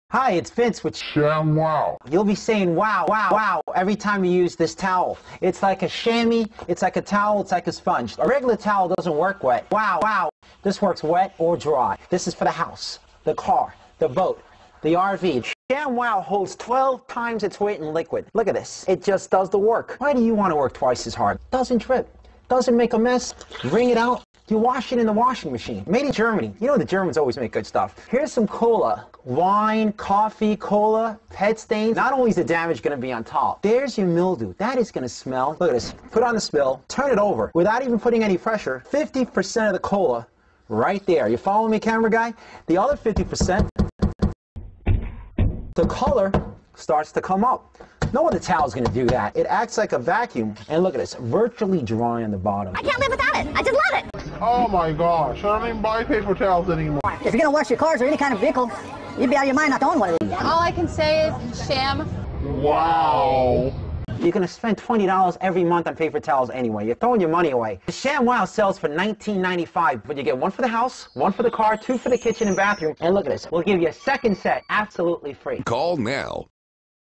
Shamwow Remix
Shamwow commercial that I edited a bit.